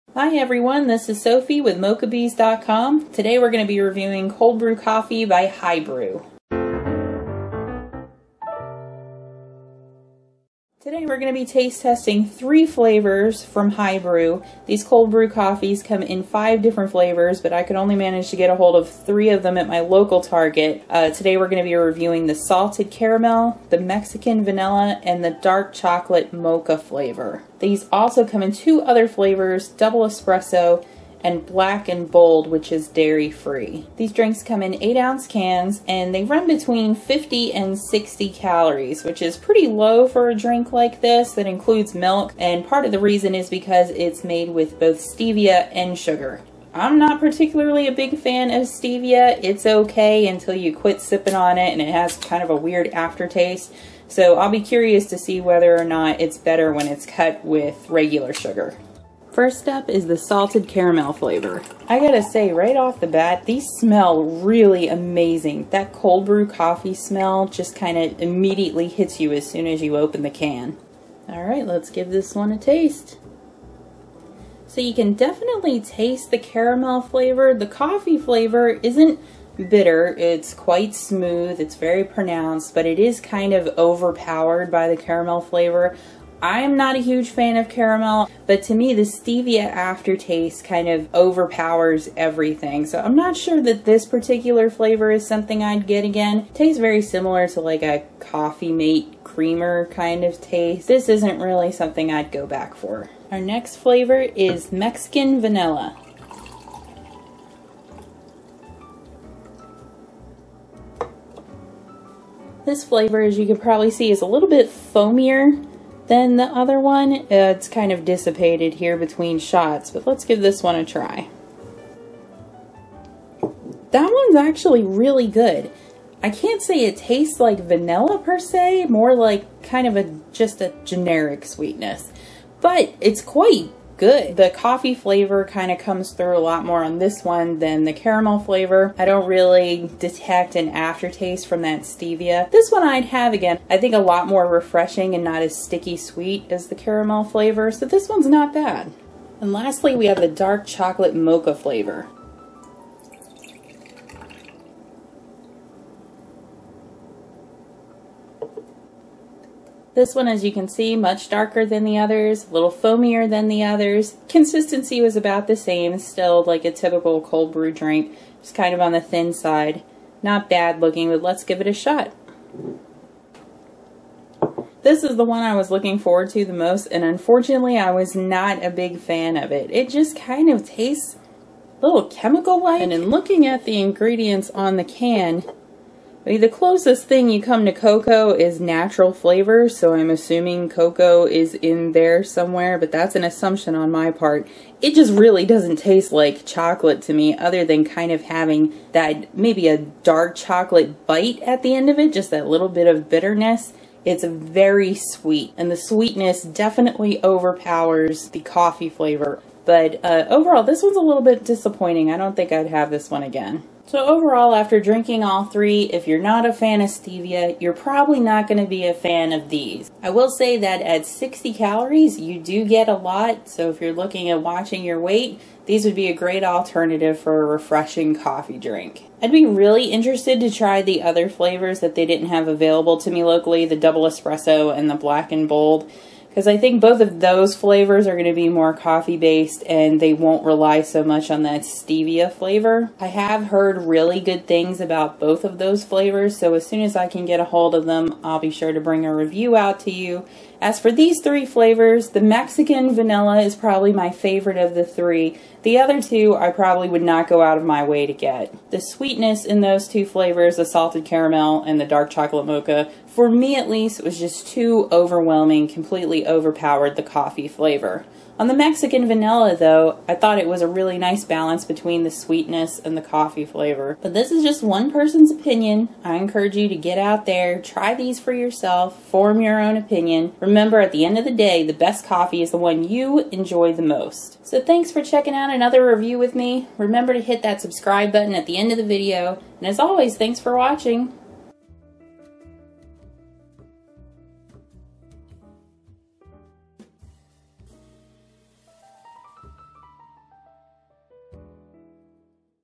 High Brew Cold Brew Coffee - Review & Taste Test
We taste test three Cold Brew Coffee flavors from High Brew Coffee, based in Austin, Texas:

- Salted Caramel
- Mexican Vanilla
- Dark Chocolate Mocha

These canned cold brew coffees were purchased at my local Target store. Two other flavors are also available, but were not for sale at my local store.

These cold brew coffees are unique in that they're sweetened with both sugar and stevia. They're ~50 to 60 calories per 8 oz can.